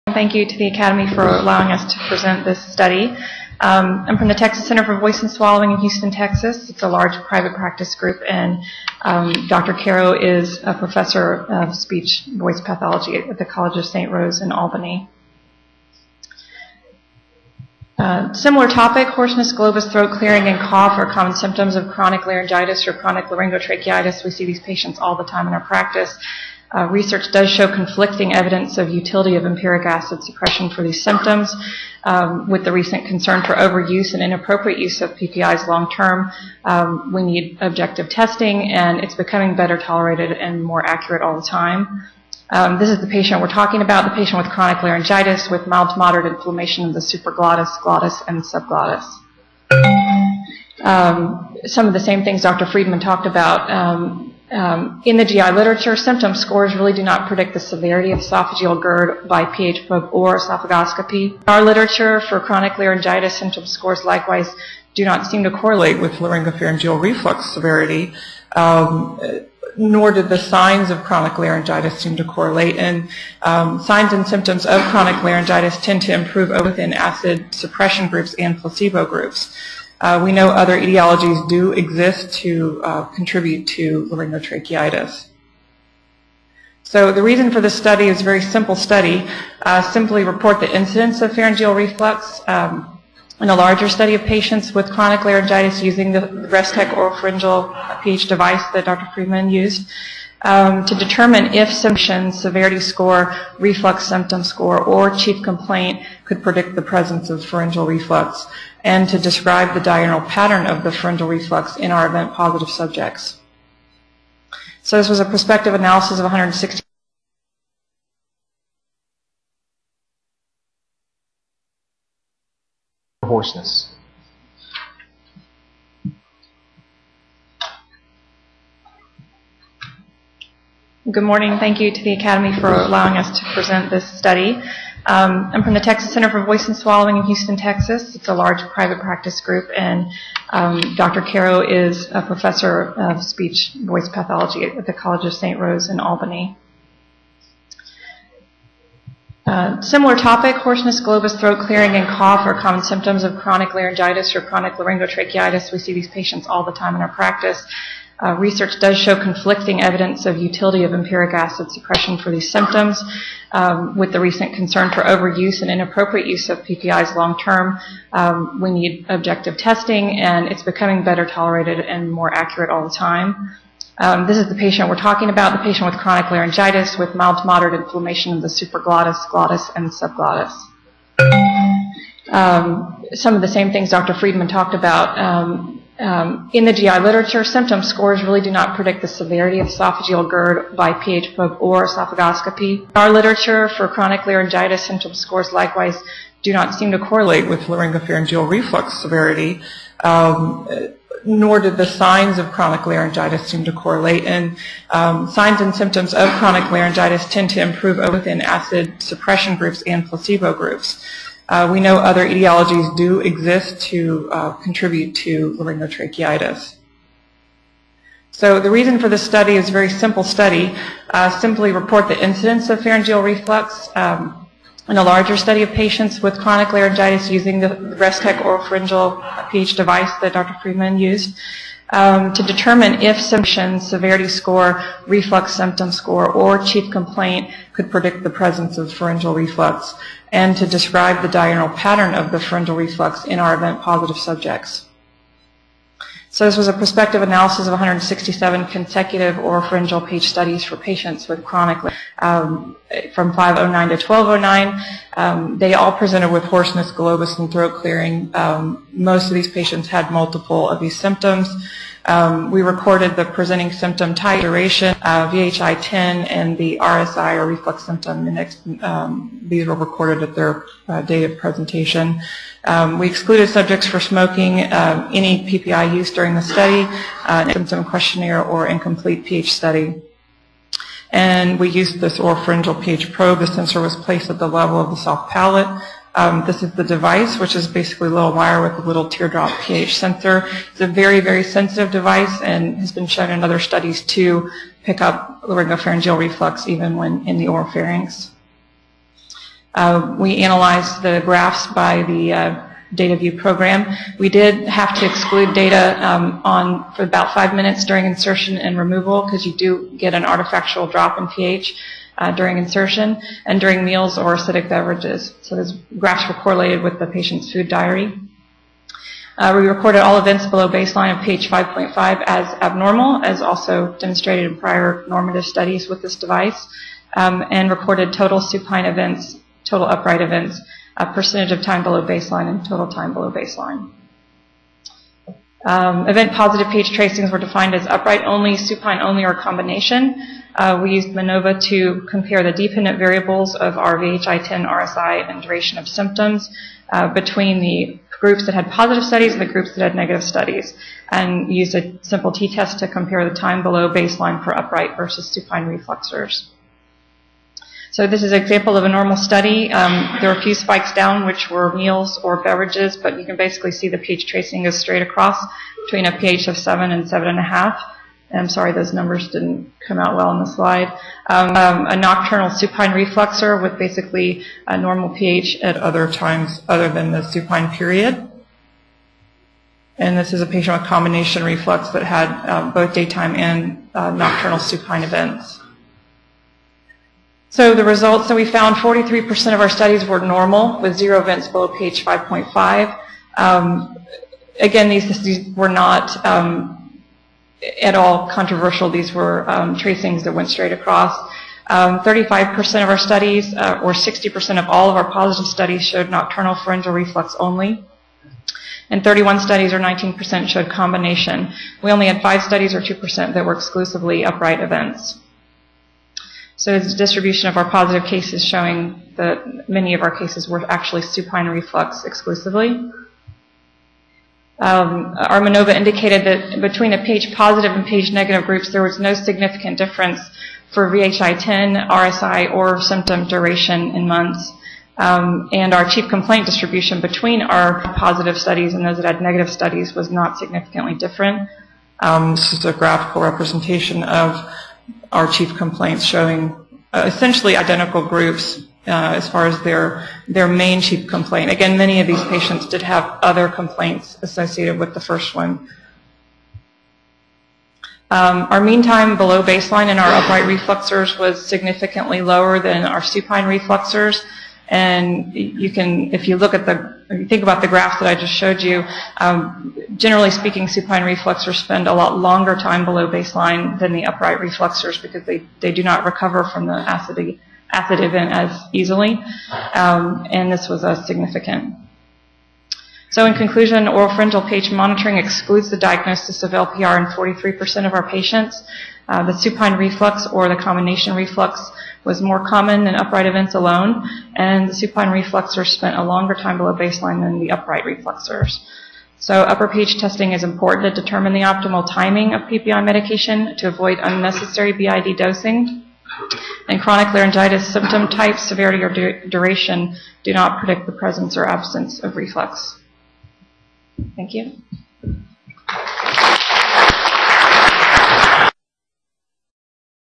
Dr. Mary Es Beaver, Laryngologist Restech LPR Study
Dr. Beaver discusses the use of Restech to definatively diagnose patients presenting with symptoms suggestive of acid reflux.  Dr. Beaver has over five years of clinical and research experience with Restech's Dx-pH probe and is an expert in pharyngeal pH measurement.